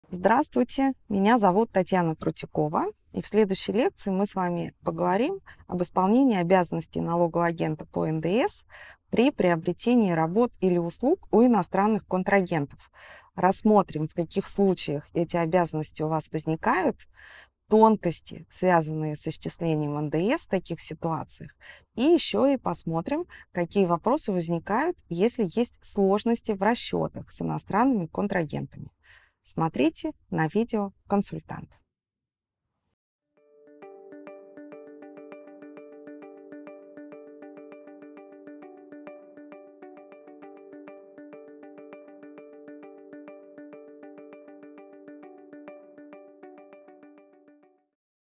Уплата НДС налоговым агентом при покупке у иностранного контрагента | Смотрите на Видео.Консультант
Лектор расскажет об исполнении обязанности налогового агента по НДС при приобретении товаров (работ, услуг) у иностранного контрагента. Разъяснит, в каких случаях такие обязанности возникают, а в каких – нет. Особое внимание уделит практическим примерам.
Лектор: Крутякова Татьяна Леонидовна, аттестованный преподаватель ИПБ России, автор многочисленных книг и публикаций в профессиональных бухгалтерских изданиях.